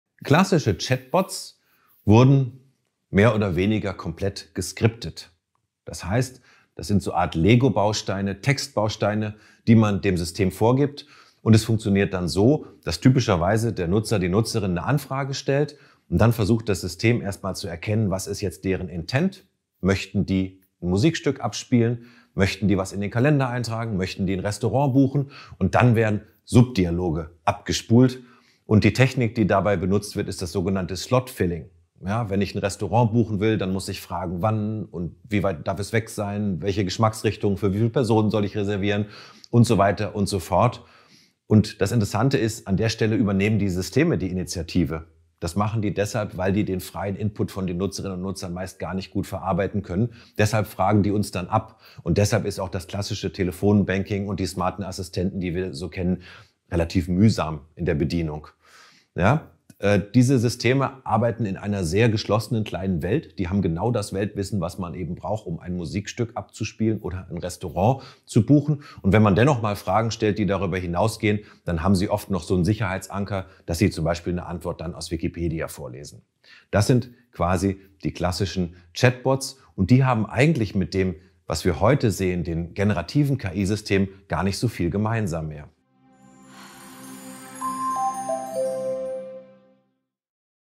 Wie funktionieren klassische Chatbots?
Aljoscha Burchardt (DFKI) erklärt, wie klassische Chatbots funktionieren.

Das Video stammt aus dem ersten Modul des Online-Kurses "Sprachassistenzen als Chance für die Hochschullehre", in dem es um die Grundlagen von KI-basierten Sprachmodellen geht. In dem Kurs lernt ihr, KI-Sprachmodelle produktiv, aber auch kritisch reflektiert an der Hochschule einzusetzen.

Der KI-Campus ist ein vom Bundesministerium für Bildung und Forschung (BMBF) gefördertes F&E-Projekt. Er ist seit Juli 2020 als Beta-Version öffentlich verfügbar.